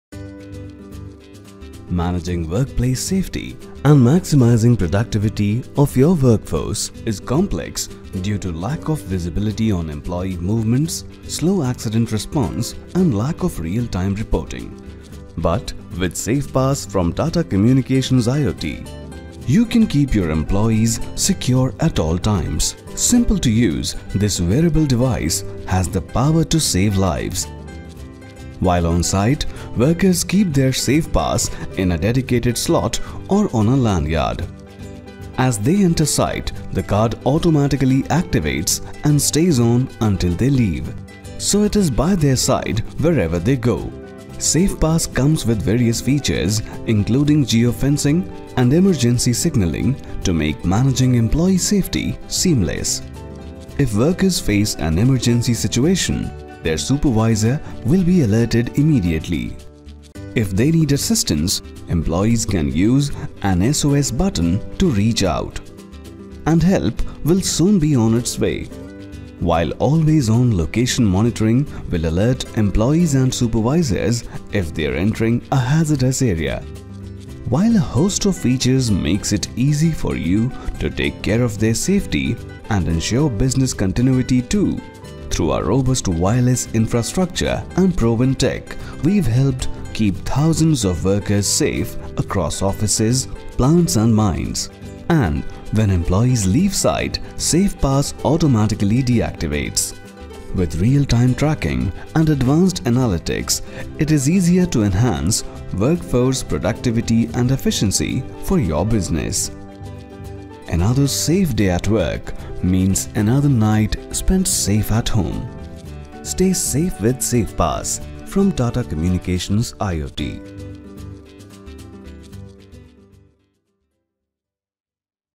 IoT Connected Worker Solution | SAFEPASS | Tata Communications
Enterprises with a large number of workers find it difficult to maintain optimal levels of visibility on the workers, which may lead to loss of productivity or the workers ending up in situations where they need urgent help. Tata Communications brings to you a solution that uses IoT for workforce productivity.

Safepass® is an IoT solution that needs to be carried on the person of workers at all times whiles within the work premises. It has two features- One is the SOS alert button, which sends an alert to the worker's supervisors for three seconds to obtain urgent help. The second function is real-time location monitoring and hazardous zone warnings.

Such IoT solutions increase worker productivity, safety and security, and satisfaction. Tata Communications also provide IoT connectivity management, and all devices can be monitored from the command centre. Safepass® serves as an IoT connectivity platform unifying the workforce and its supervision from a single command centre.